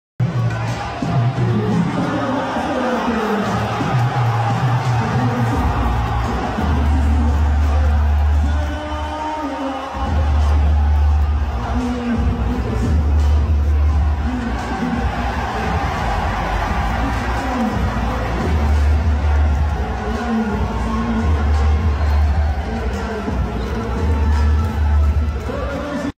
He's Back! John Wall unleashes the Dougie in LA
It's been a long time since we've seen John Wall hit the dougie